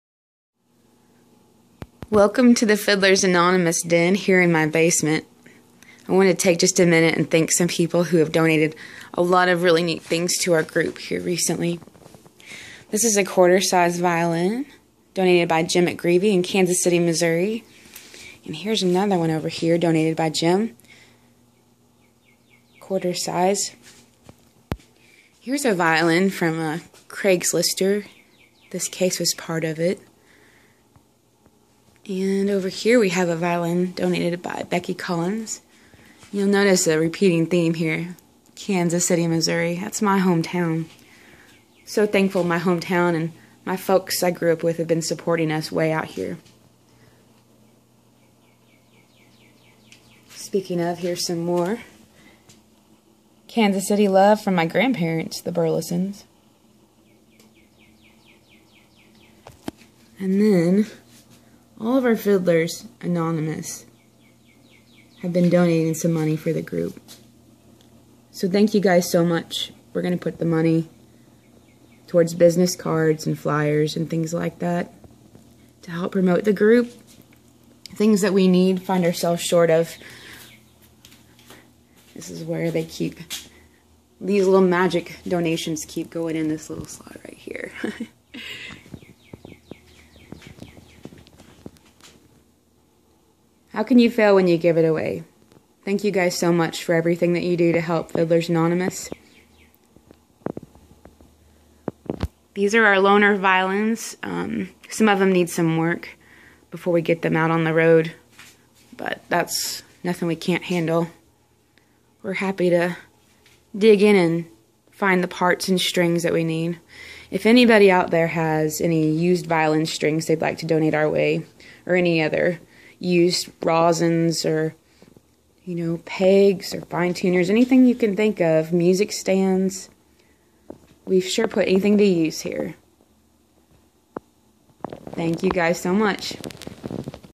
A word of thanks!
We have been gifted some neat things recently, folks are showing our little rag tag gang of fiddlers an awful lot of generosity. We want to thank you all!!